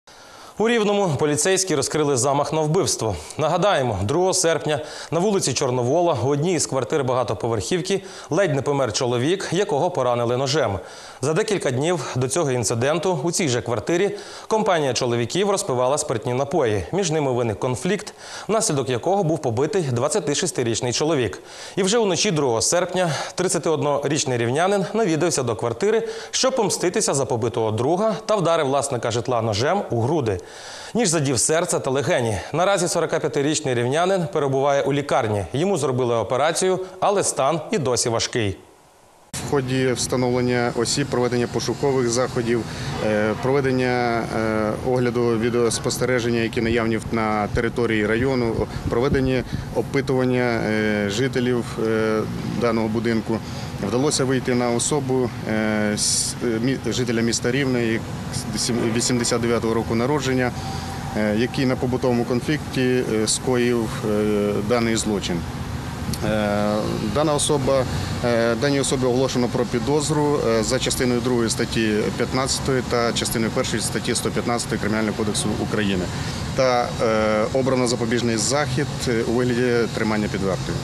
У Рівному поліцейські розкрили замах на вбивство
Нагадаємо, 2 серпня на вулиці Чорновола у одній з квартир багатоповерхівки ледь не помер чоловік, якого поранили ножем. За декілька днів до цього інциденту, у цій же квартирі, компанія чоловіків розпивала спиртні напої. Між ними виник конфлікт, внаслідок якого був побитий 26-ти річний чоловік. І вже у ночі 2 серпня, 31-но річний рівнянин навідався до квартири, щоб помститися за побитого друга. Та вдарив власника житла ножем у груди. Ніж задів серце та легені. Наразі 45-ти річний рівнянин перебуває у лікарні. Йому зробили операцію, але стан і досі важкий.